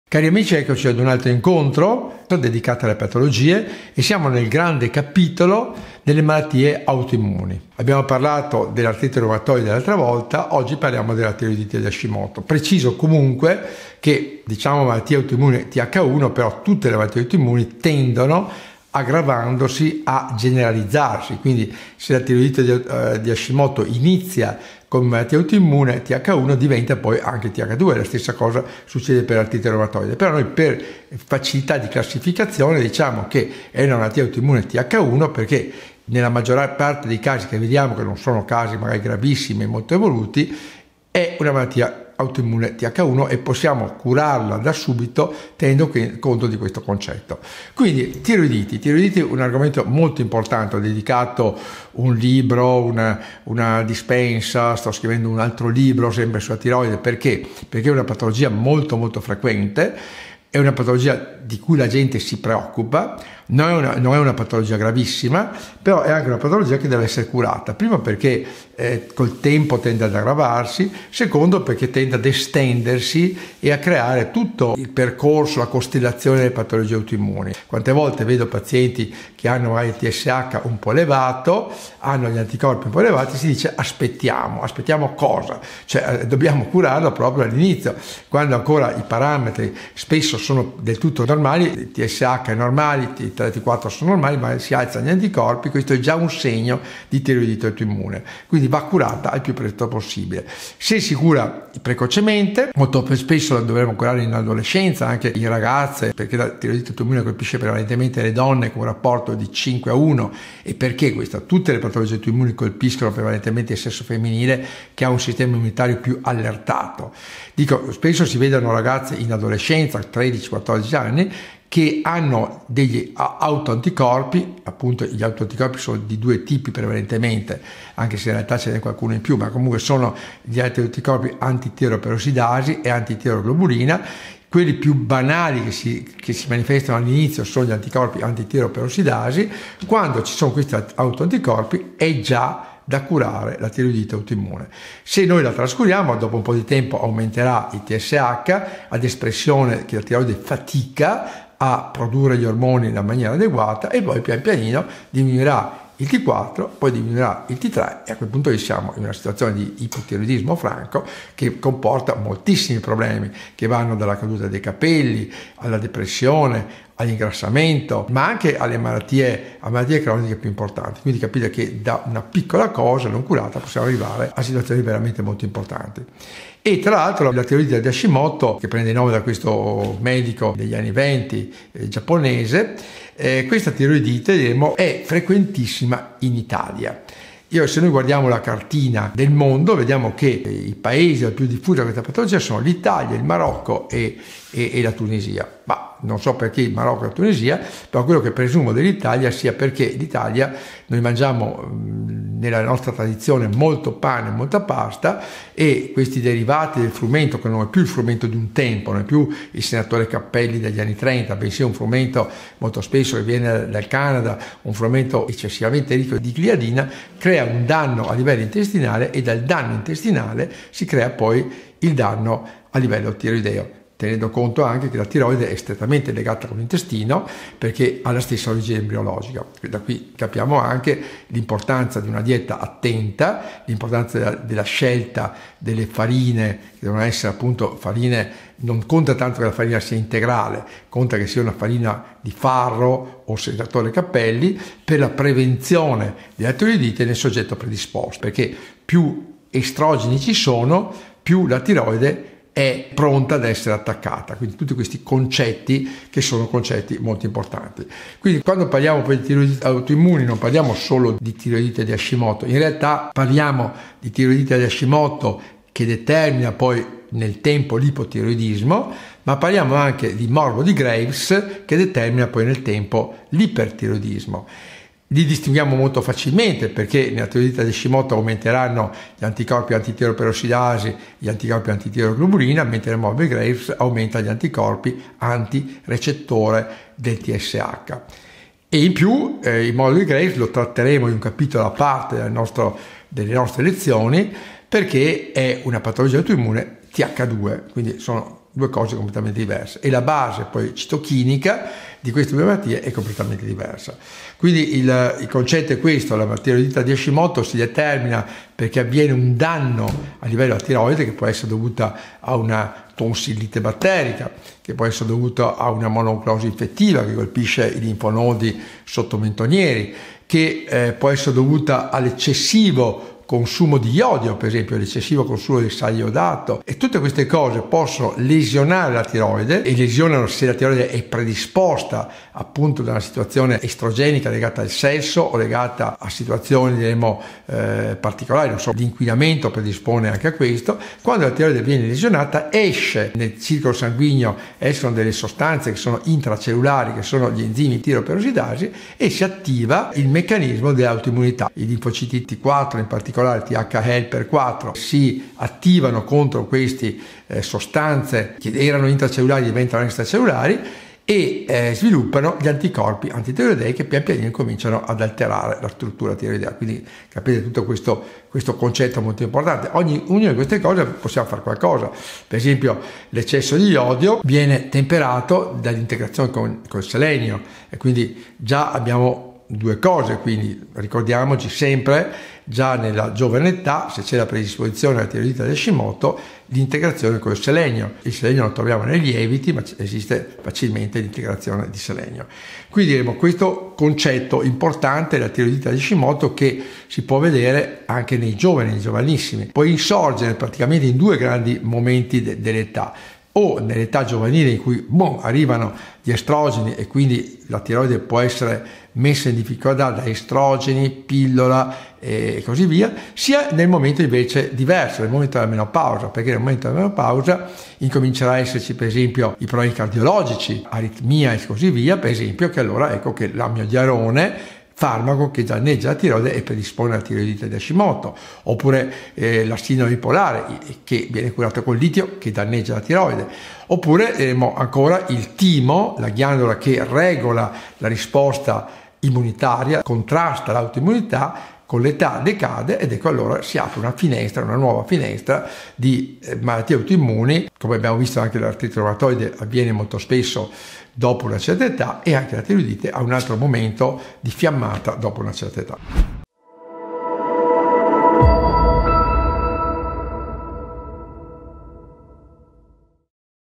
Caduta dei capelli e tiroidite di hashimoto
Oggi il Dott. Bianchi ci parlerà di una possibile causa per la caduta dei capelli o depressione, la Tiroidite di Hashimoto

- Scopri la Masterclass del Dott. Ivo Bianchi! Clicca qui per maggiori info

- Vuoi una Consulenza online con il Dott. Ivo Bianchi? Clicca qui per maggiori info

Arrivederci in salute con la Natura. Al prossimo video!